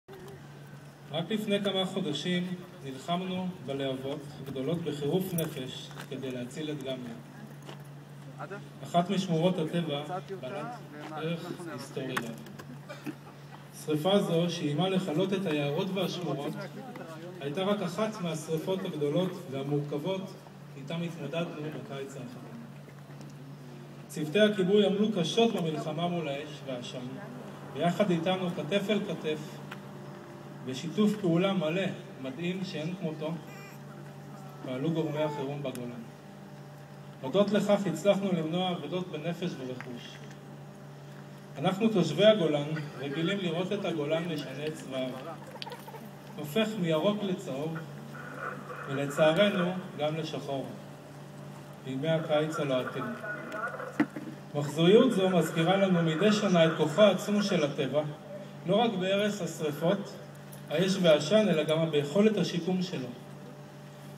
פתיחת תערוכת הצדעה לכבאי ישראל "אש ועשן" בקצרין II
The opening of "Fire & Smoke" exhibit @ Katzrin, Ramat Hagolan
on Monday, March 14th 2011.
Artists in honor of the Israeli Firemen